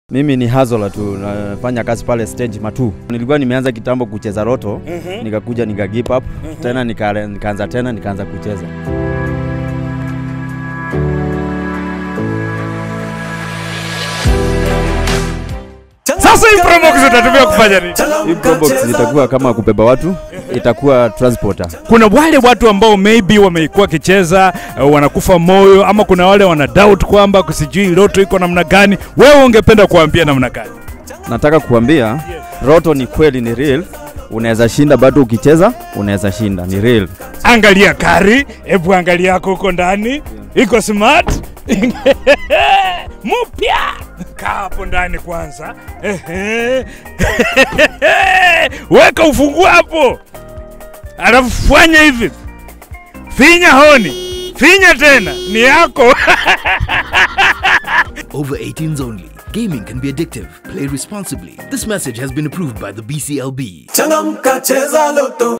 LOTTO GOES DEEP INTO KAMBALAND
Daniel Makau was our lucky Lotto winner and drove away with a Brenda New Toyota Probox 2016 Model. He will venture into the transport business and operate on the Matuu - Masinga route.

You too can win big prizes with LOTTO:
PAYBILL: 777000
ACC: WEB
AMOUNT: 50 - 1K